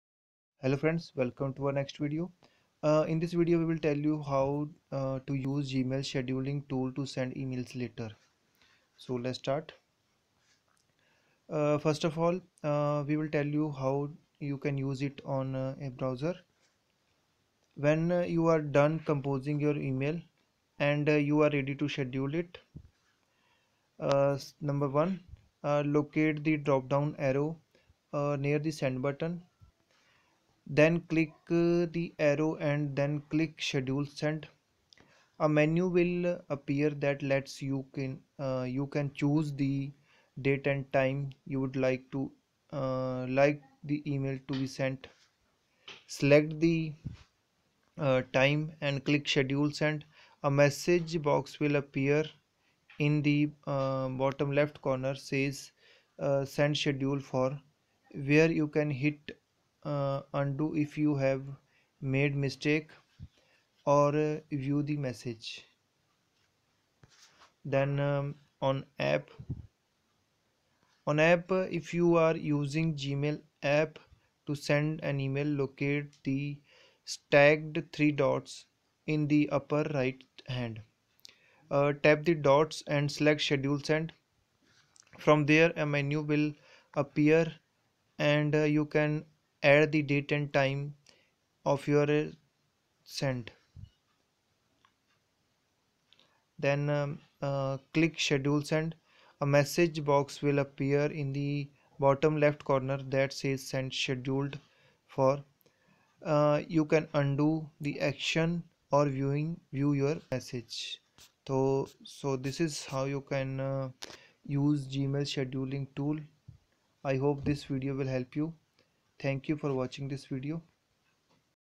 How to use Gmail's scheduling tool to send emails later
Gmail's scheduling tool is here. In this video we will tell how you can use this tool on Gmail App and on browser.